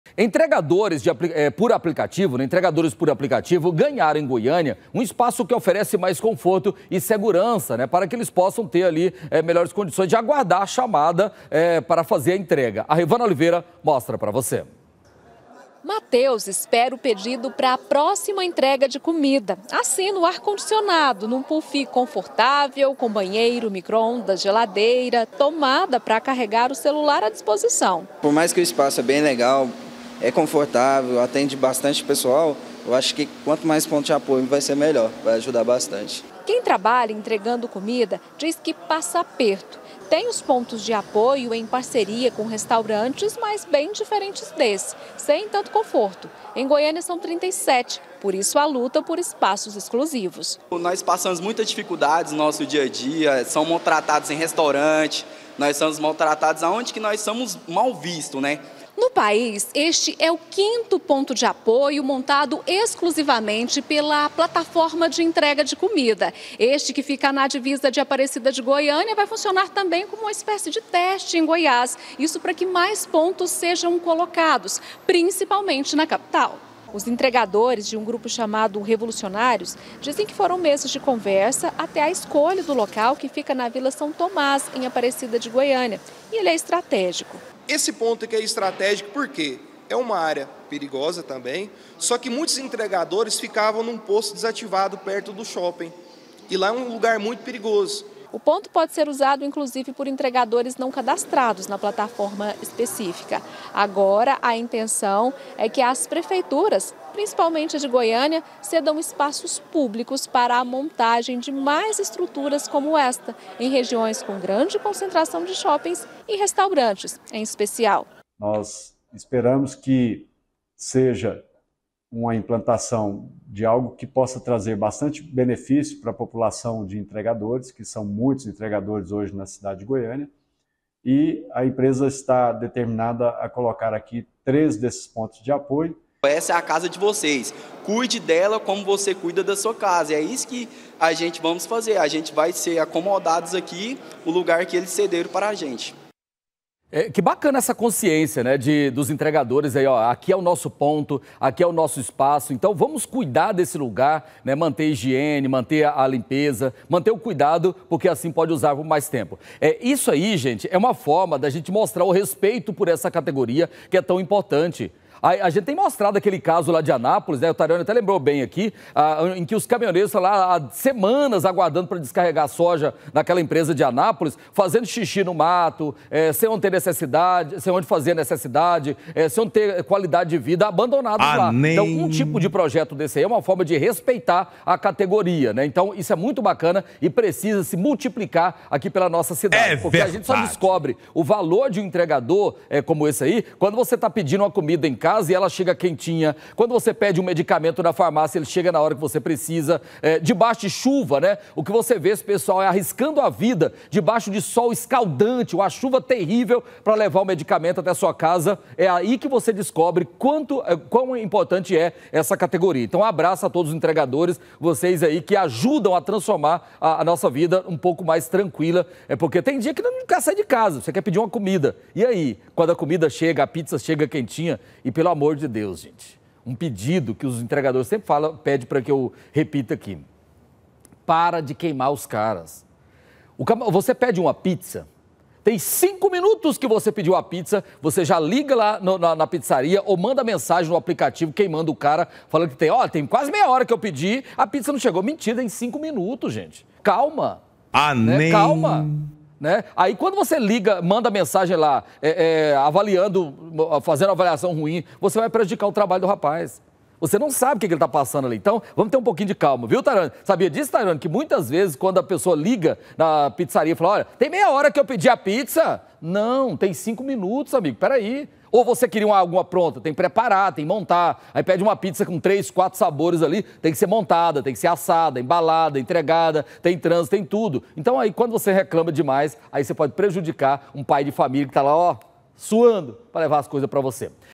PONTO DE APOIO: MAIS SEGURANÇA E CONFORTO PARA ENTREGADORES
ENTREGADORES DE APLICATIVO GANHARAM EM GOIÂNIA UM ESPAÇO QUE OFERECE MAIS CONFORTO E SEGURANÇA PARA ESSES TRABALHADORES.